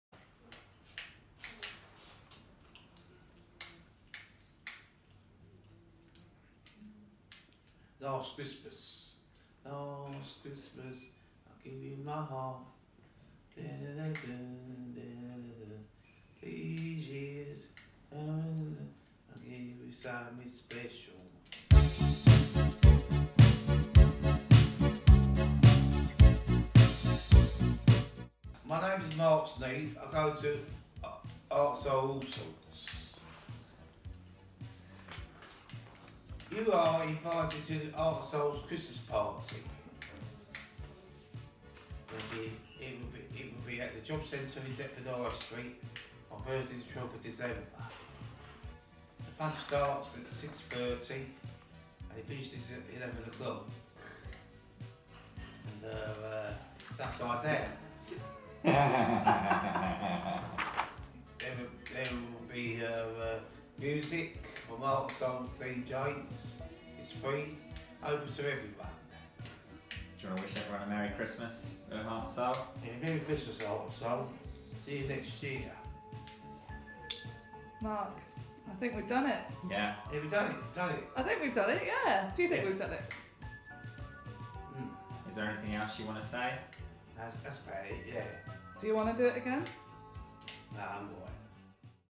Mark Snead's Christmas Party Invite
Mark Snead invites you to Heart n Soul's annual Christmas party!
When: 6 - 11pm, Thursday 12 December
Where: The Job Centre, Deptford High Street
Cost: Free
Open to everyone
Mark Snead: guaranteed